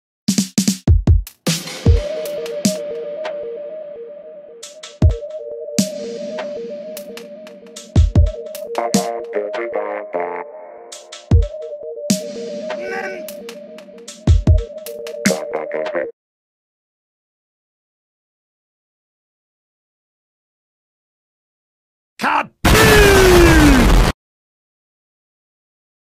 [Splatoon GMOD] What is that? (FILLER)
BEFORE ANYONE FREAKS OUT, THIS IS JUST A FILLER ANIMATION

This little 26-second animation is me trying out a new animation style. None of the events that occur in the video is canon. This video will help me stall for my upcoming animation: Agent 8's New Life (1/2)